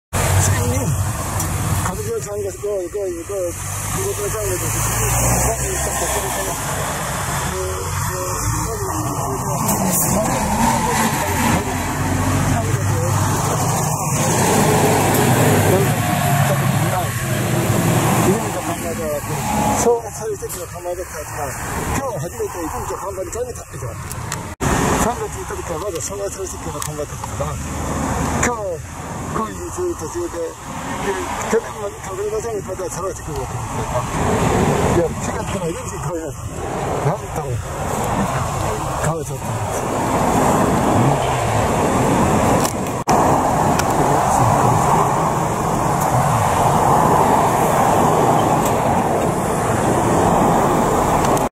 昭和シェルから出光に変わったスタンド 春日井市坂下町
２０２２年９月１４日にコロニーへ行った途中で、昭和シェル石油の看板から出光の看板に変身しました。